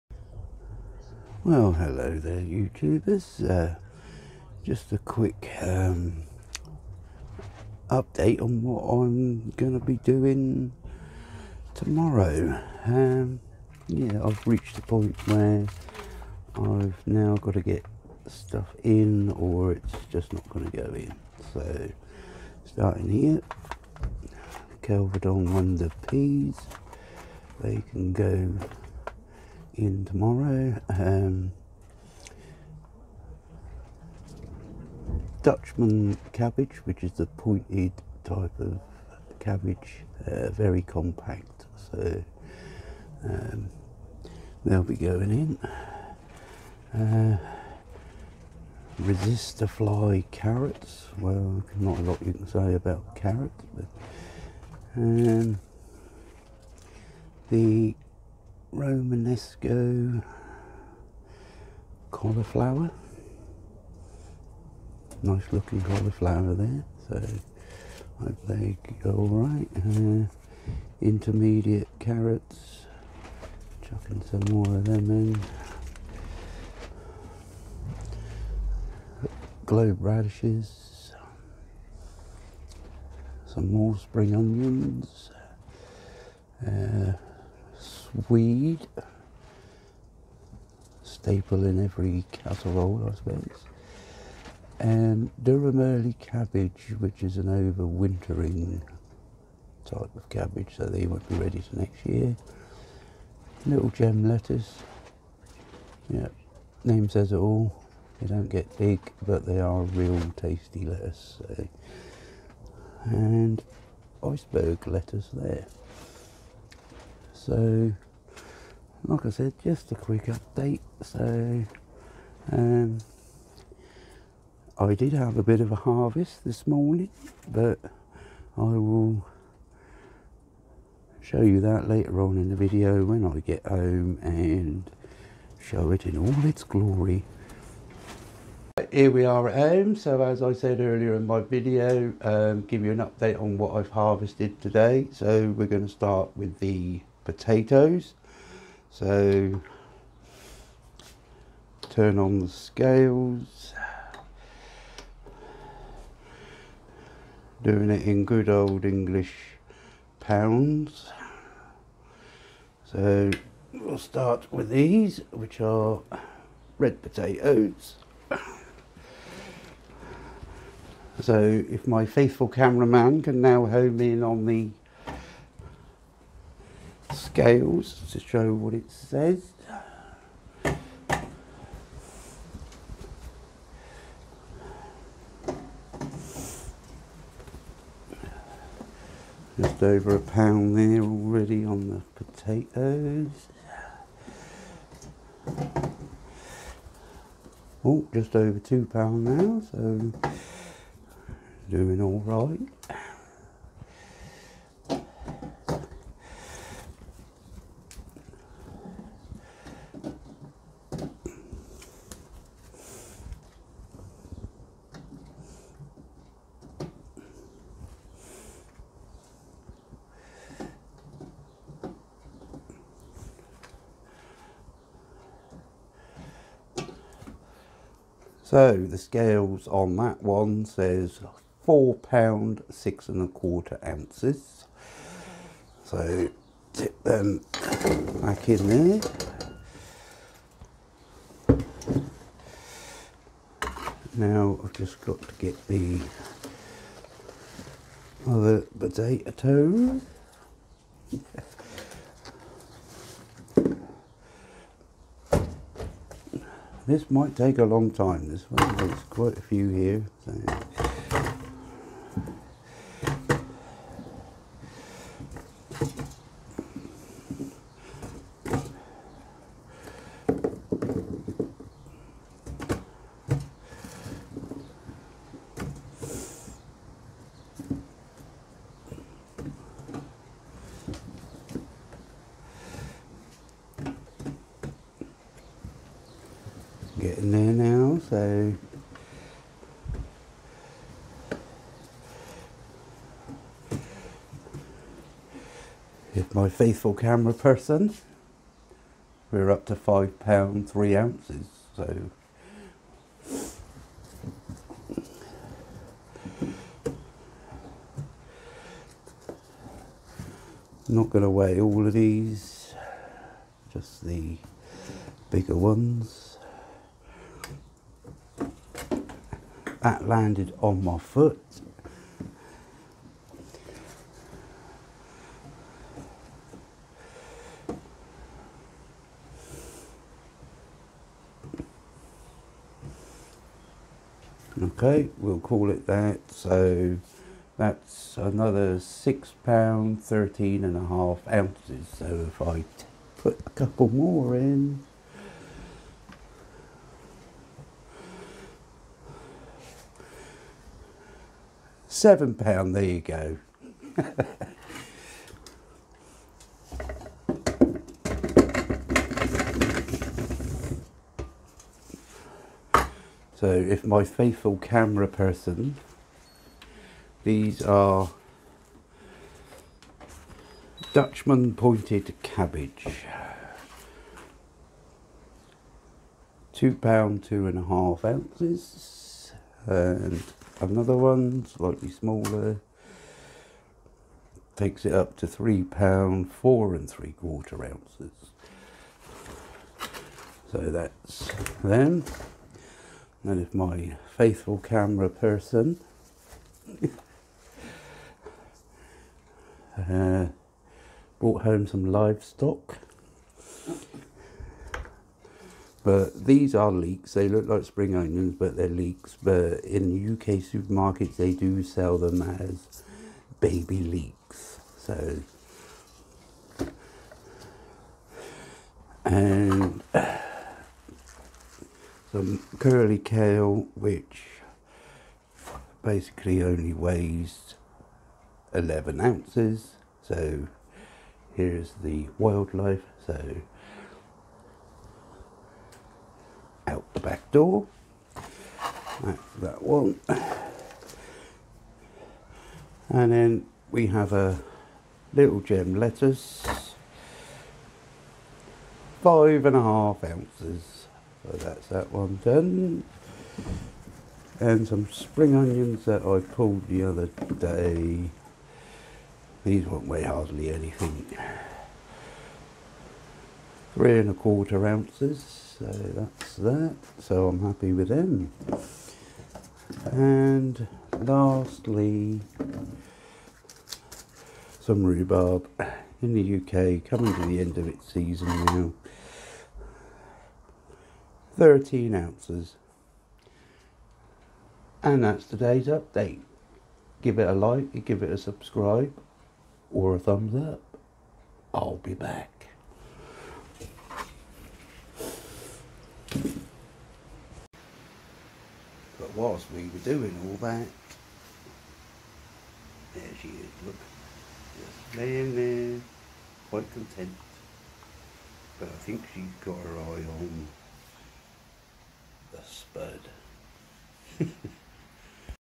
shedwars teamacrossthepond arkansaswoodcutter willitgrow teampeackeeper
A quick update on what I have to sow this week,  and a weigh in on my harvest today and my little cup cake at the end